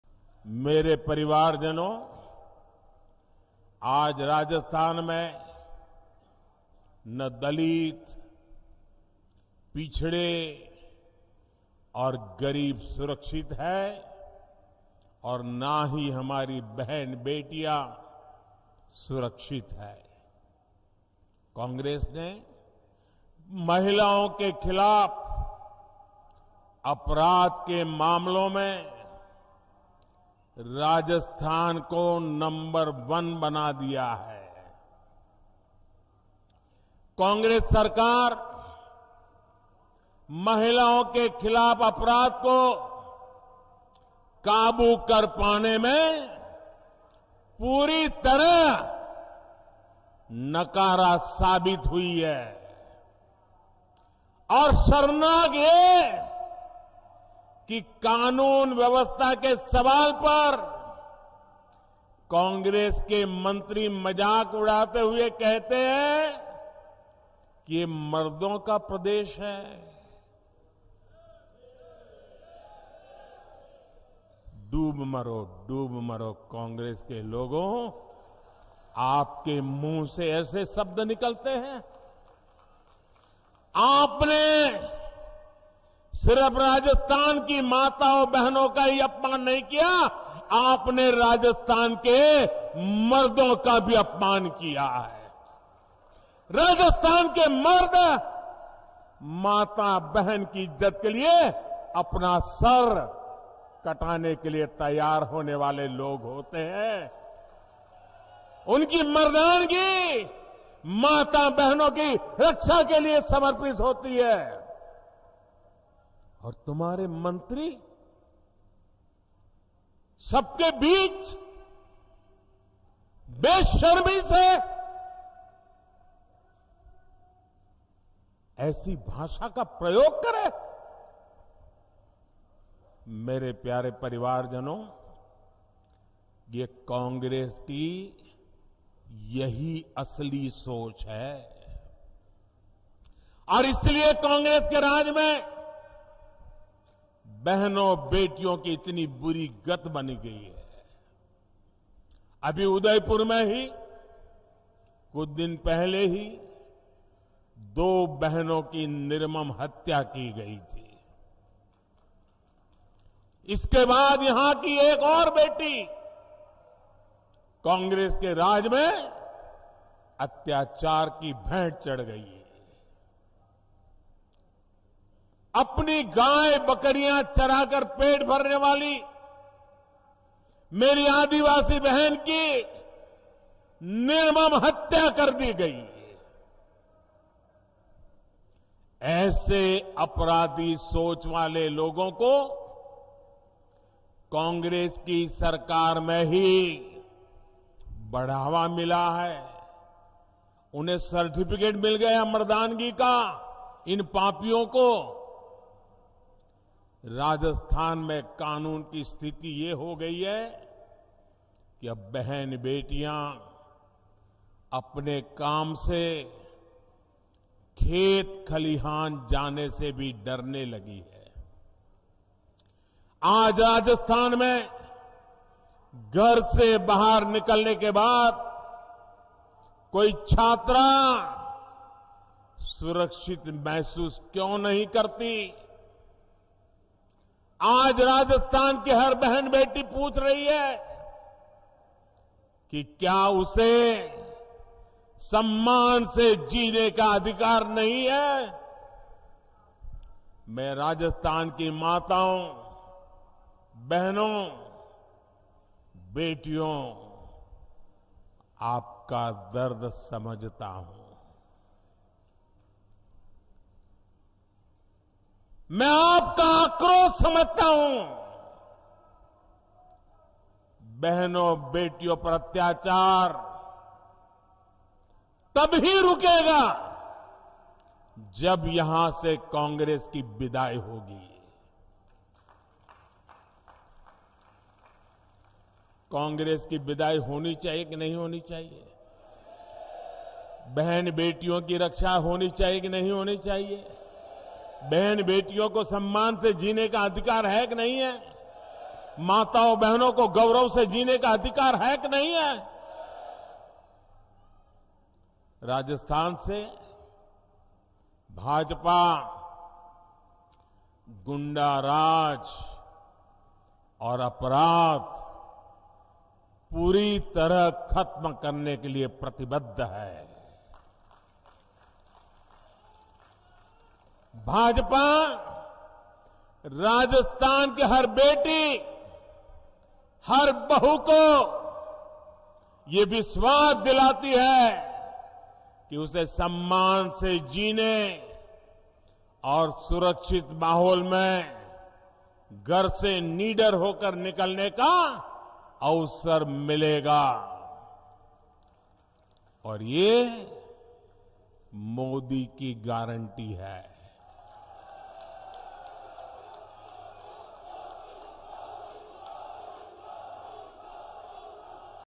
Congress राज में महिलाओं के खिलाफ अपराध में Rajasthan बना नंबर वन!
Prime Minister Narendra Modi, addressing an election rally in Udaipur, Rajasthan, said that no one is safe in Rajasthan and the Congress government has failed to control crime against women. He also criticized the Congress leaders and said that first they fail in controlling crime and then they mock the dignity of our mothers and sisters, saying Rajasthan is a men's province. PM Modi further said that he understands the pain and anger of the mothers, sisters and daughters of Rajasthan and assures them that the BJP government will eliminate hooliganism and crime from the state after coming to power.